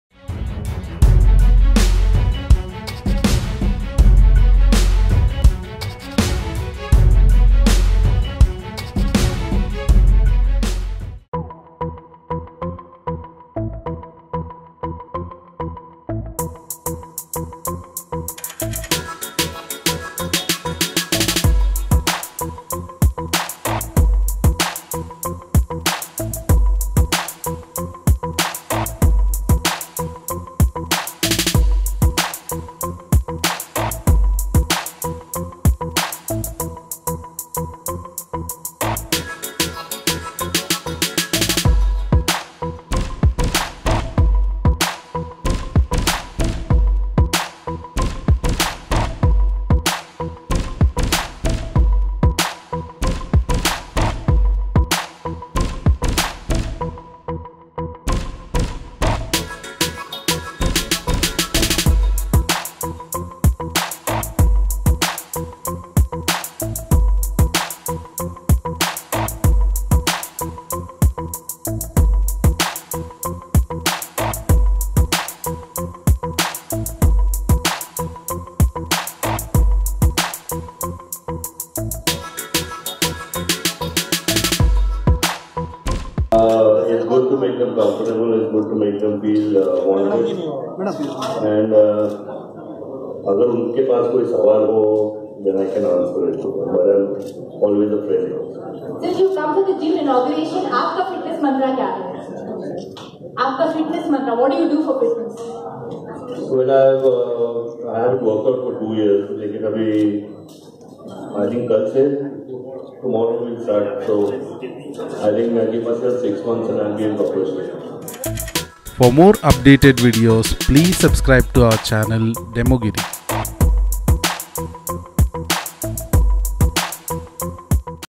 Sanjay Dutt visited Banglore - Adapt Fitness inauguration
Bollywood Actor Mr. Sanjay Dutt has recently visited Banglore for the inauguration of Adapt Fitness as he was invited for the inauguration as a chief guest and later on was spoken to the crowd of fans about the fitness role in day to day life and also revealed his fitness secret in this video..